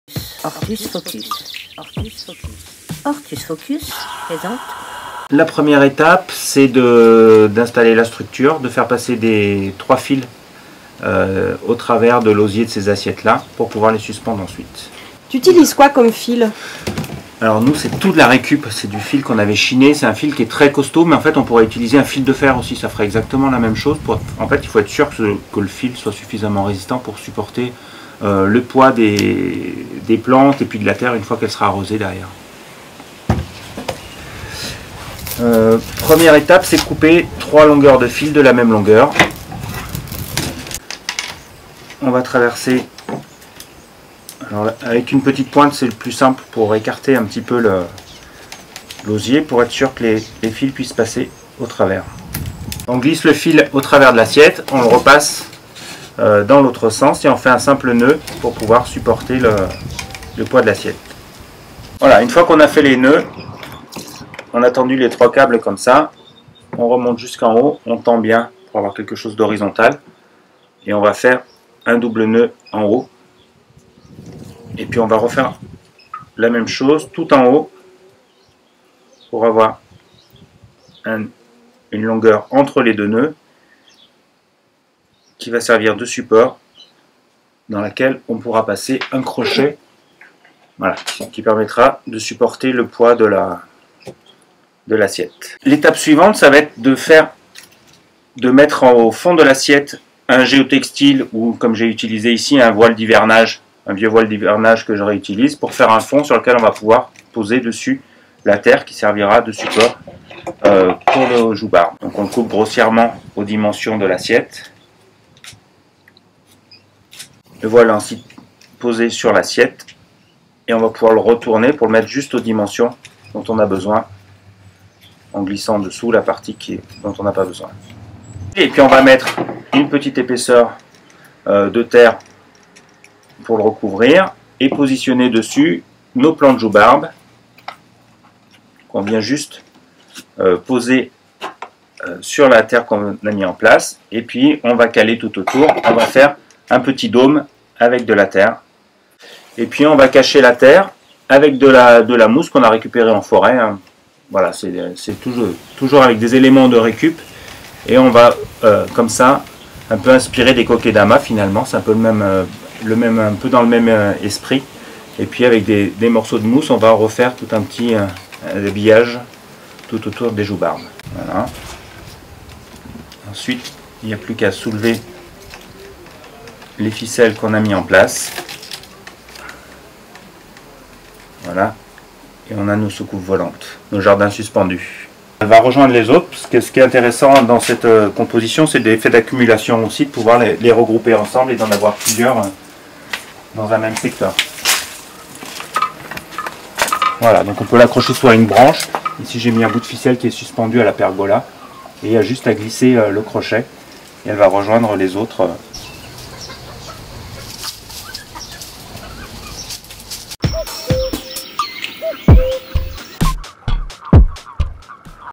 Tuto : déco, des sédums en soucoupes volantes !
Opération récup et déco, Anthony Bazin, paysagiste, montre comment créer une ambiance verte et suspendue avec des sedums dans son jardin ardéchois, le Jardin de Besignoles !
Retrouvez l'article DIY sur le magazine Hortus Focus :